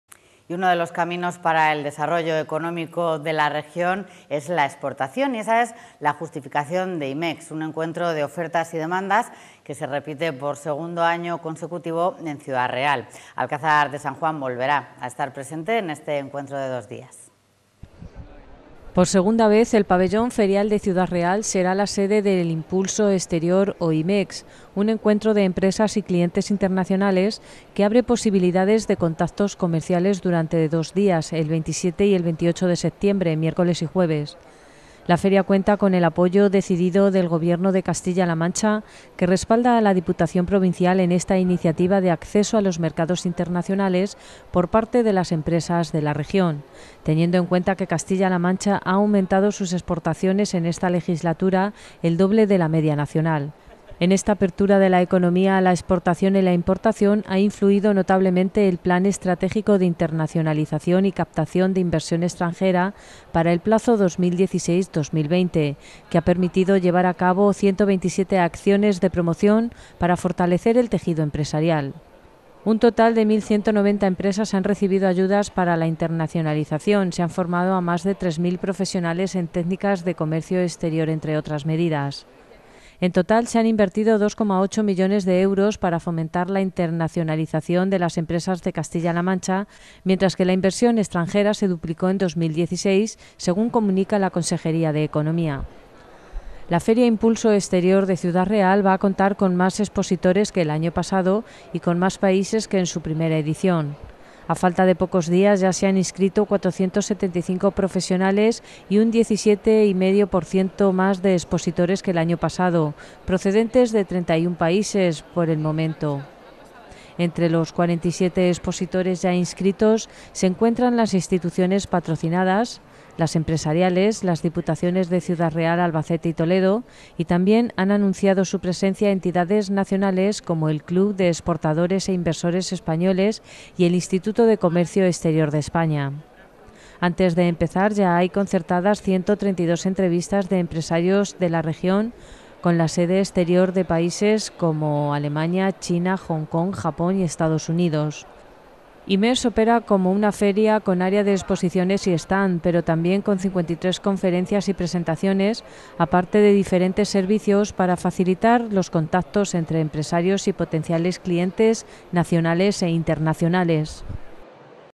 Nueva edición del IMEX
Por segunda vez el pabellón Ferial de Ciudad Real será la sede de Impulso Exterior o IMEX un encuentro de empresas y clientes internacionales que abre posibilidades de contactos comerciales durante dos dias, el 27 y 28 de septiembre